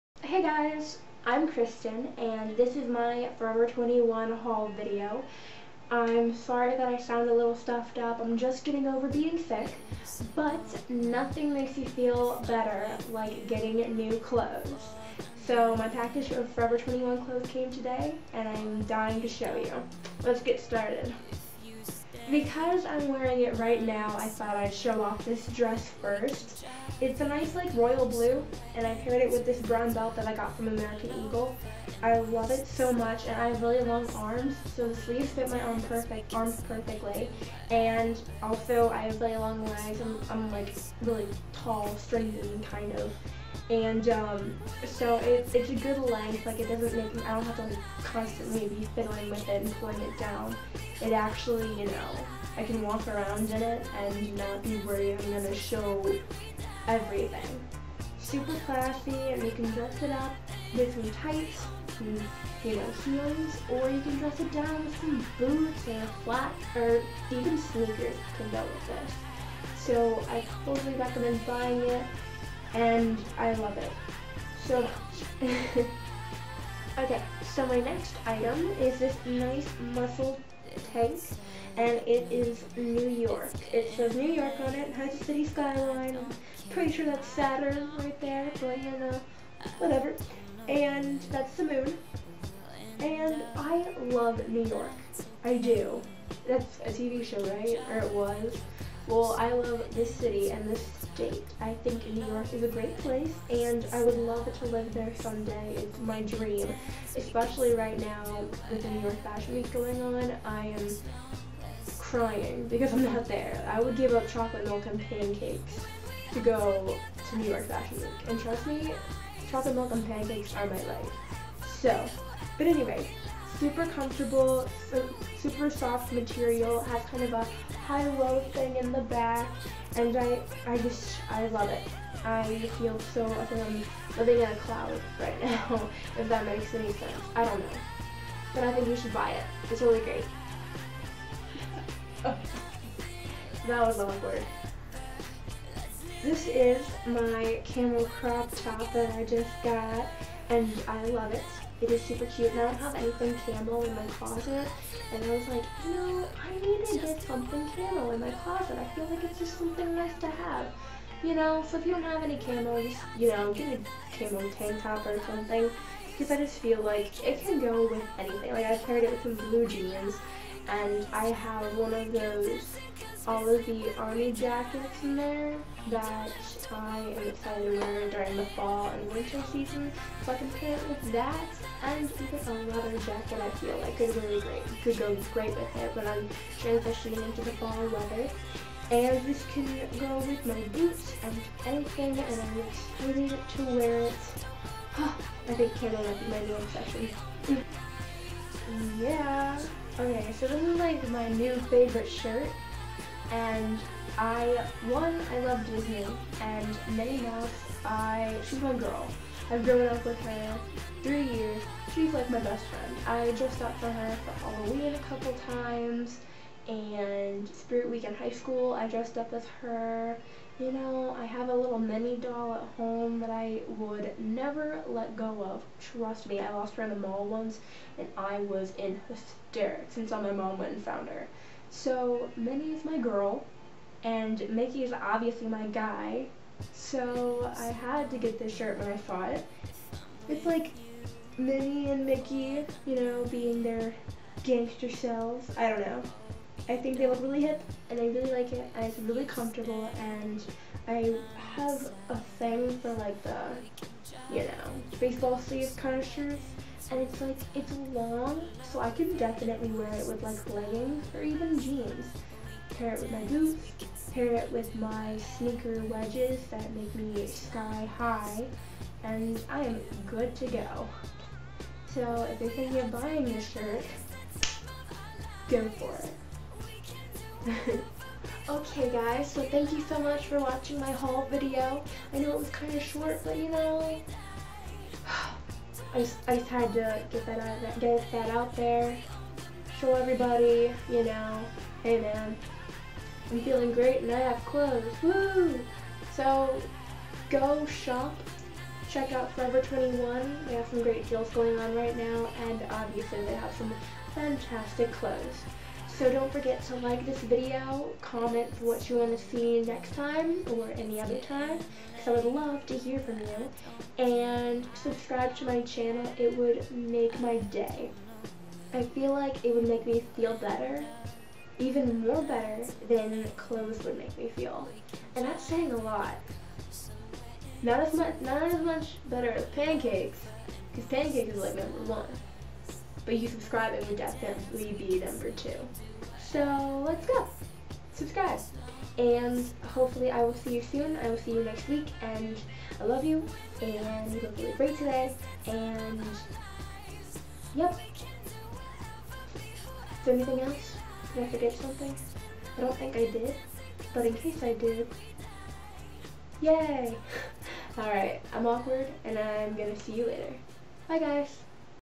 Forever 21 Haul
My clothes from Forever 21 came in the mail and I'm going to show you what I got!!